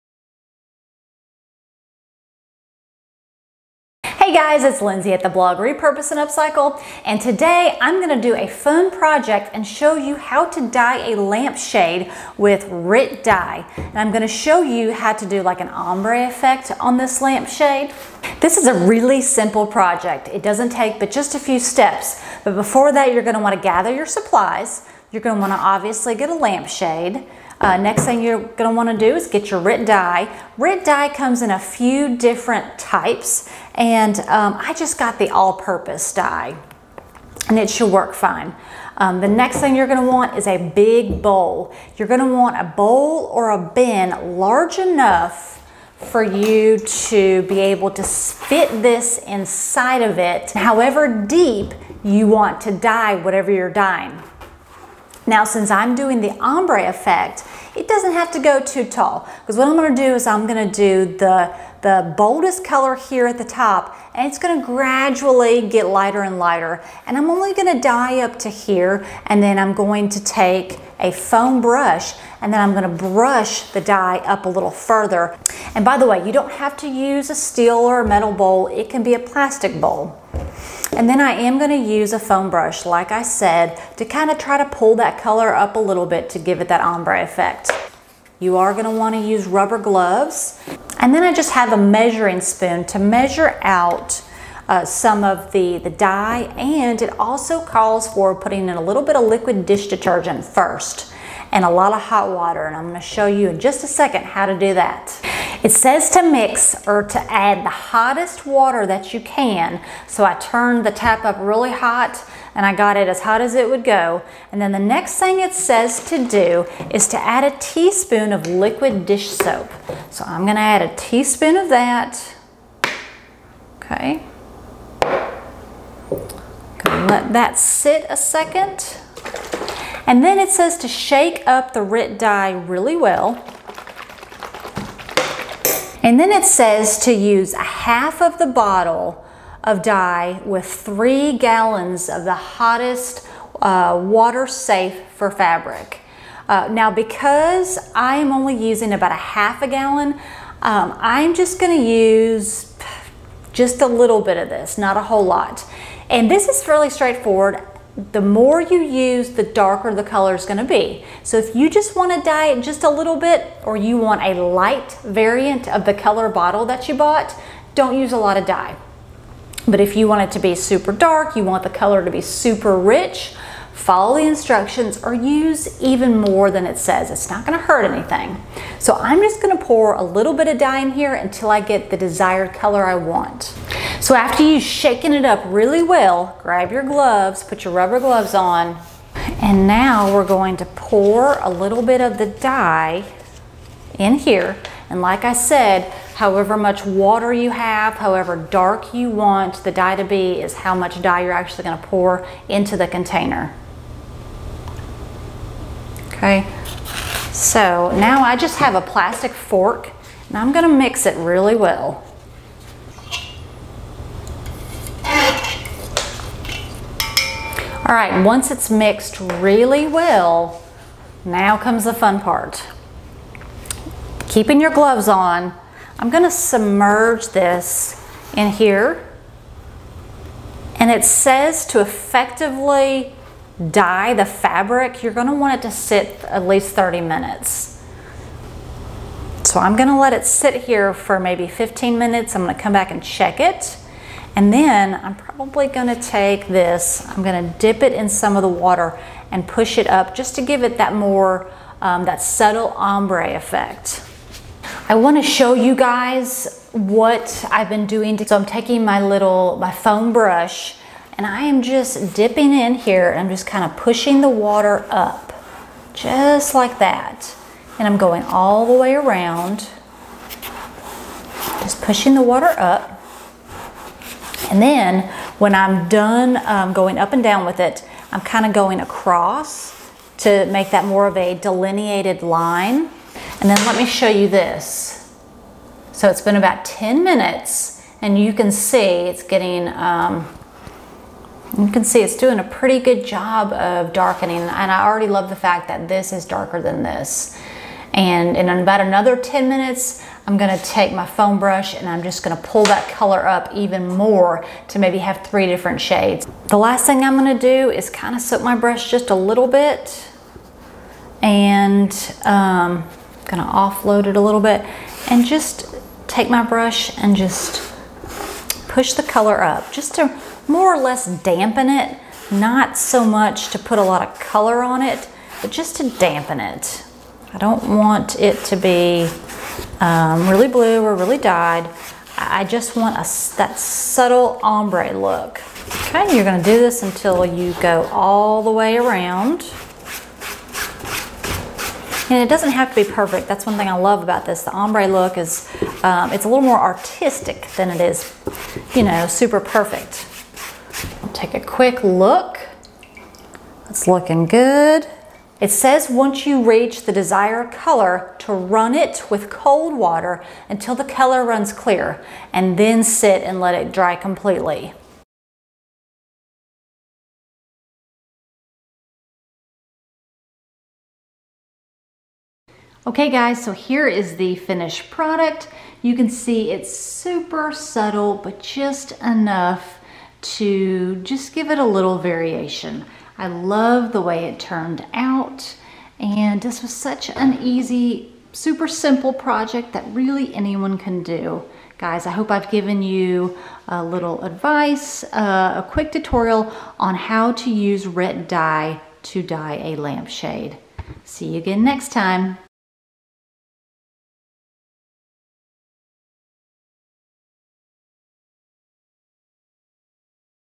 How to dye a lampshade with Rit Dye- Easy Lampshade dye project
Learn how to dye a lampshade with Rit Dye for a beautiful subtle Ombre look. This simple tutorial will show you how to use Rit Dye also. This is an easy lampshade dye project and can be completed in about an hour.